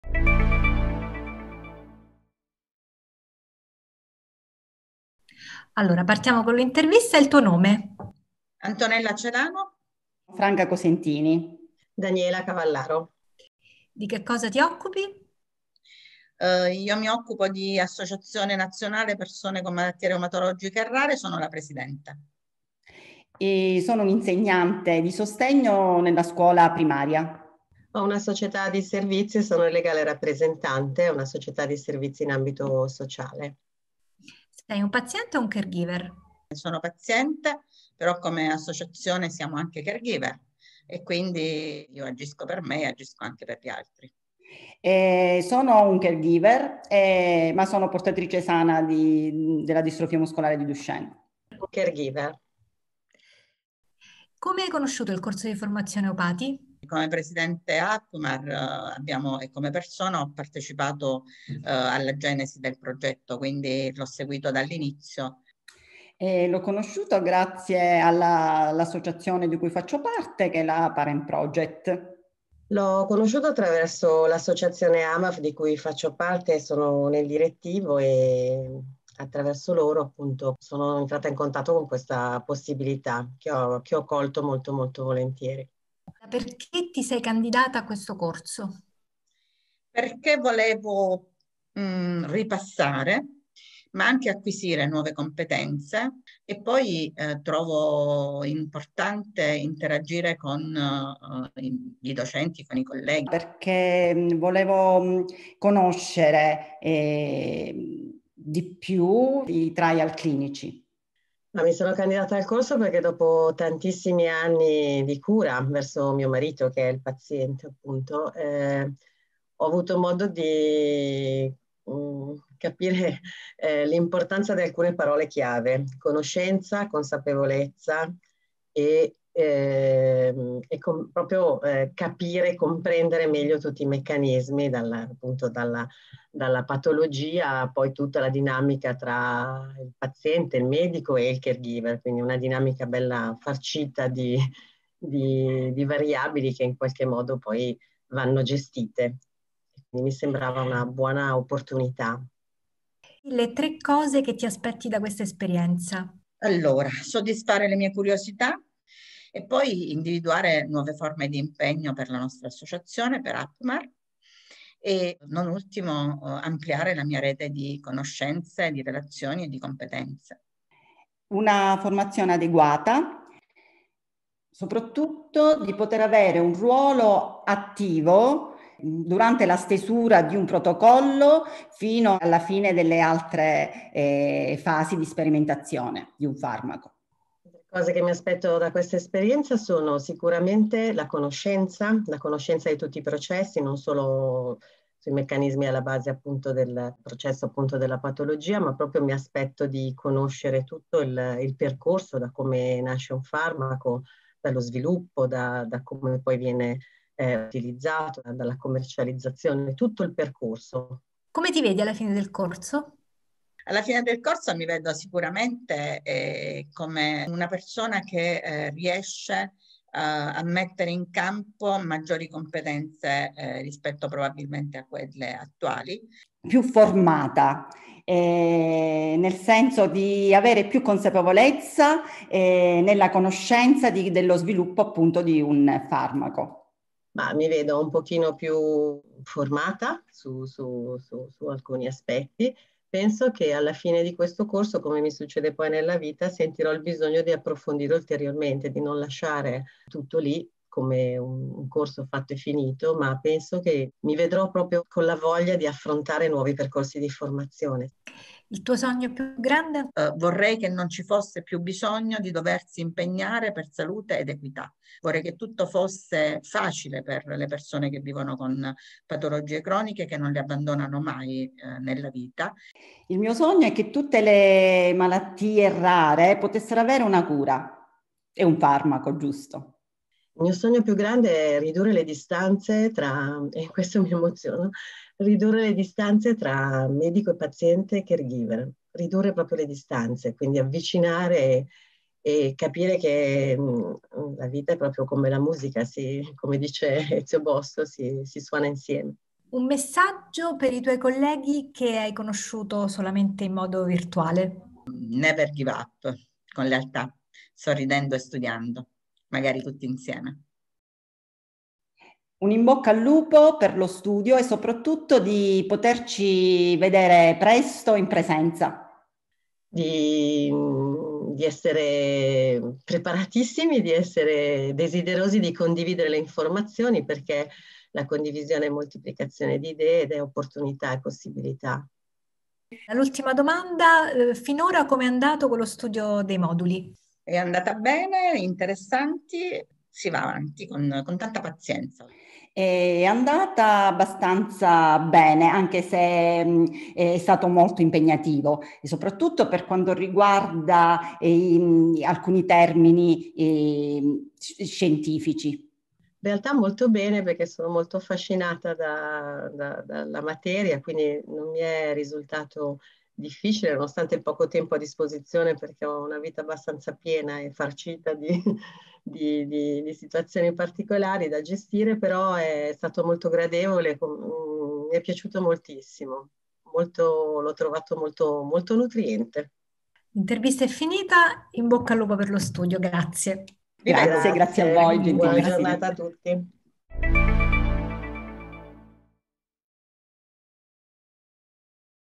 Interviste ai corsisti dell'Edizione 2020/2021 del Corso EUPATI
Intervista tripla con Antonella Celano, Franca Cosentini e Daniela Cavallaro, future Pazienti Esperte EUPATI. Cosa ne pensano gli iscritti al Corso "Patient Engagement in Ricerca e Sviluppo delle terapie innovative - Edizione 2020/2021" della formazione ADPEE.